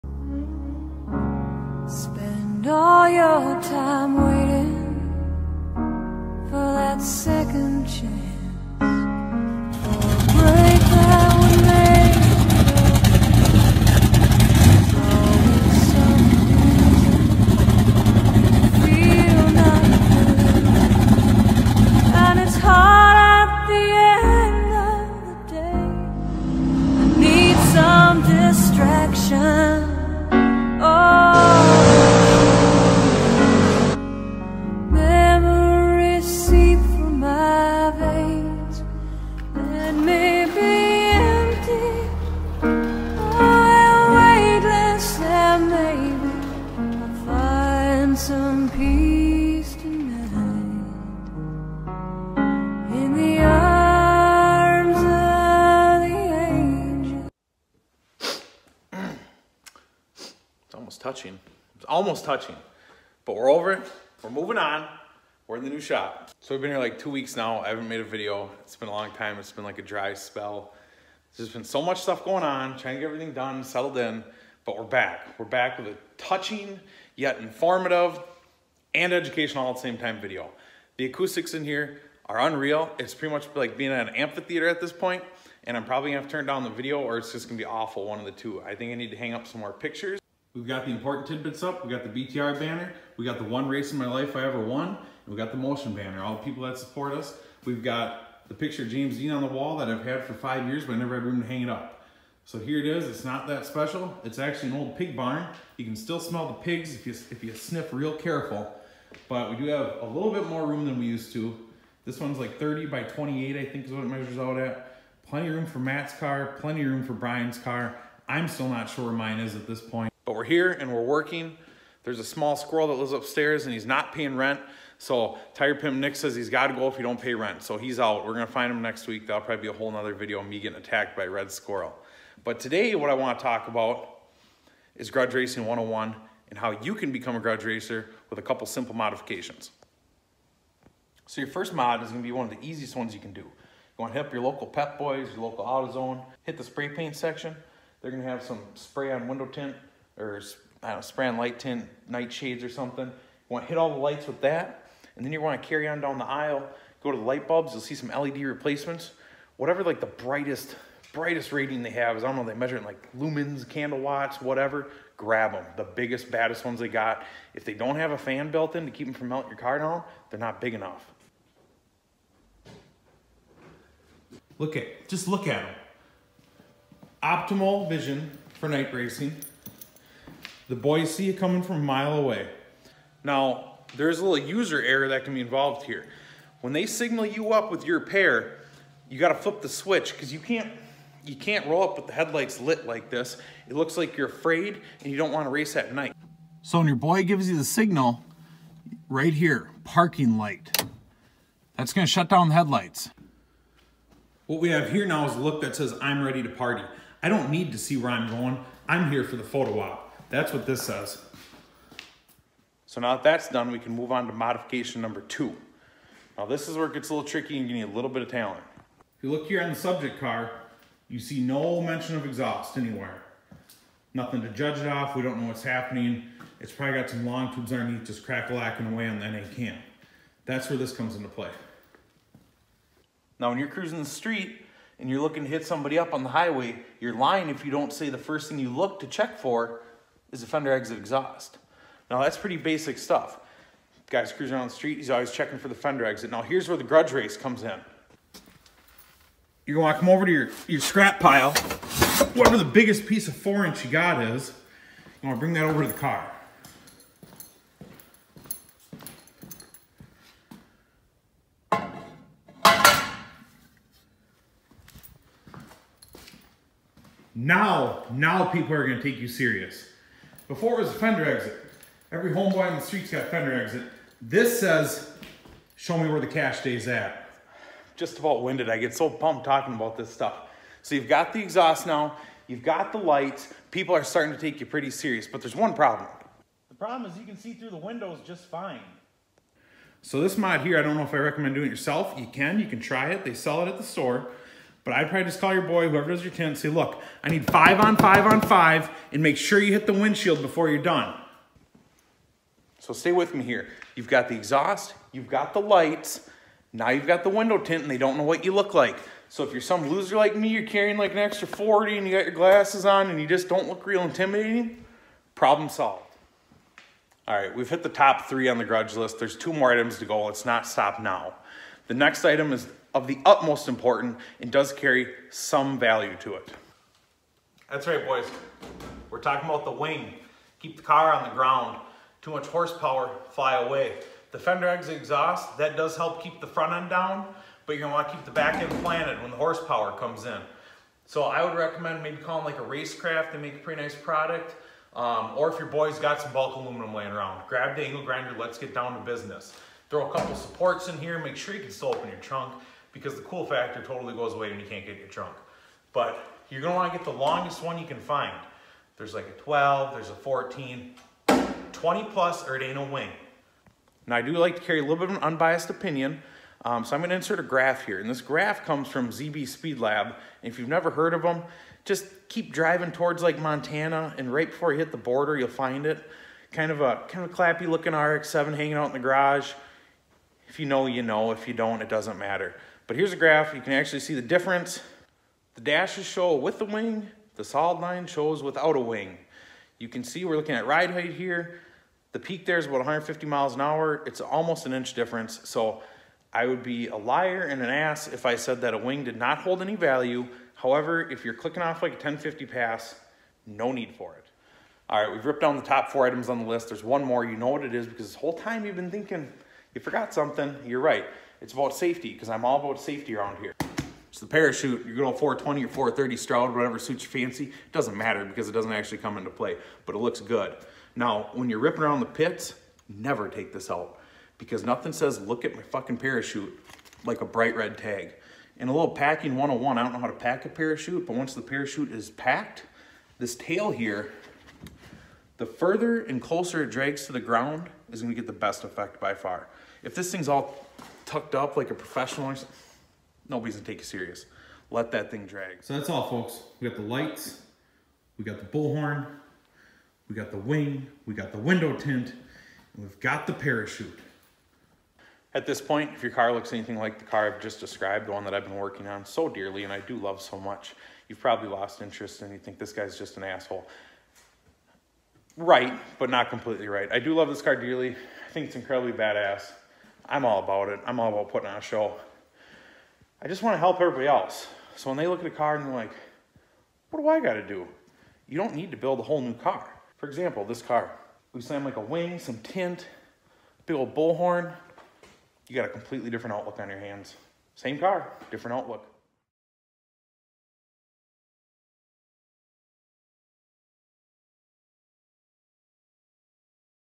How to Build a Grudge Car!
Gather round and let’s discuss how to build a proper grudge car.